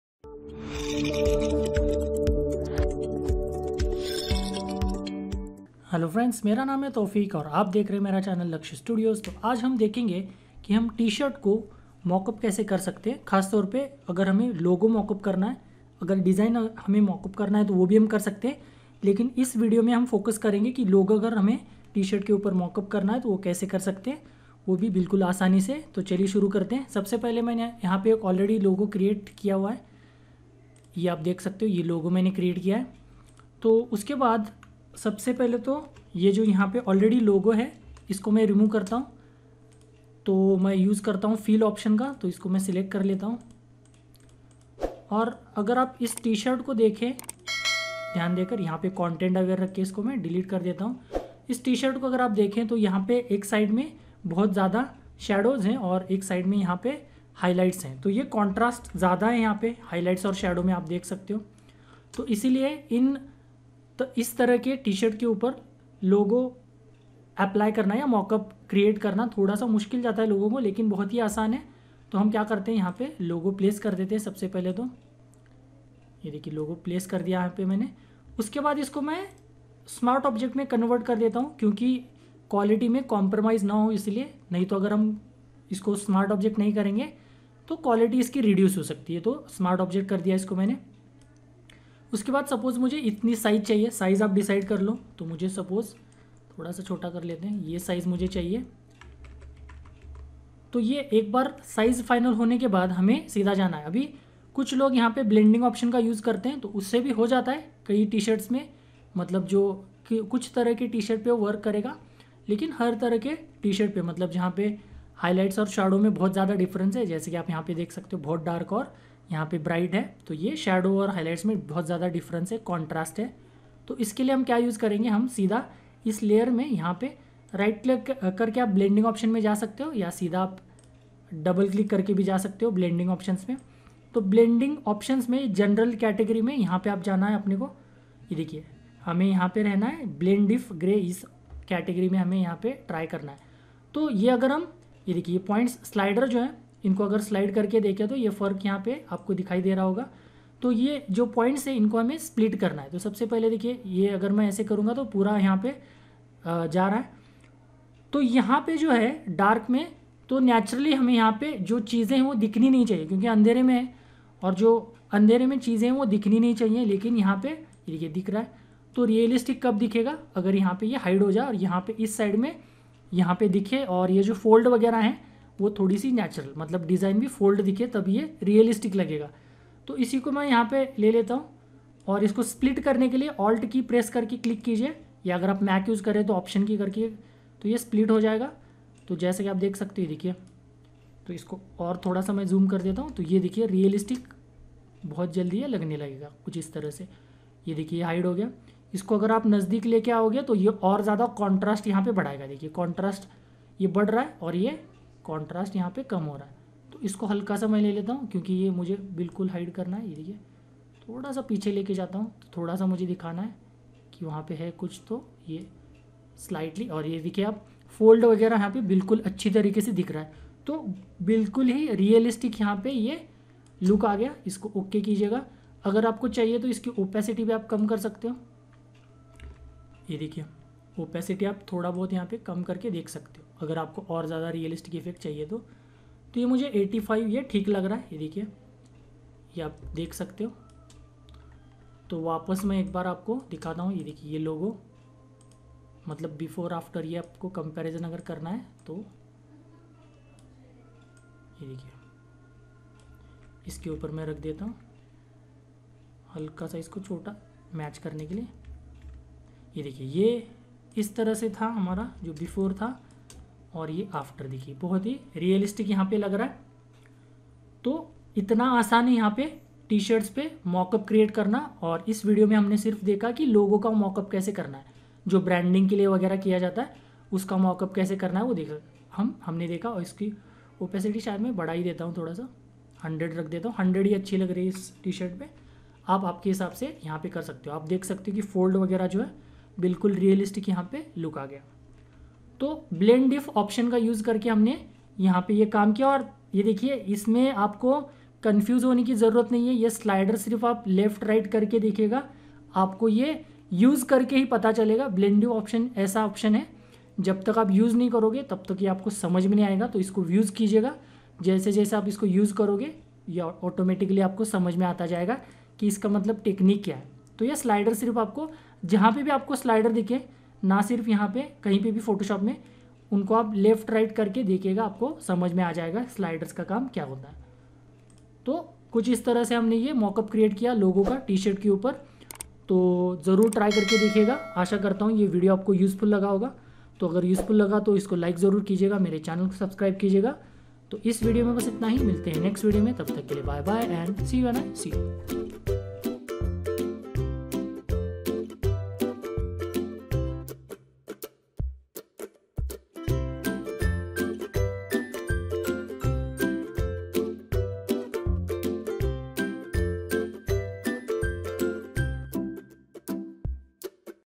Realistic T-shirt Mockup in Photoshop | Hindi
In this video we'll create a T-shirt mockup using a simple method. We'll mockup a Logo and make it as realistic as possible. This quite useful for designers as we often need Mockups after fishing the designing work.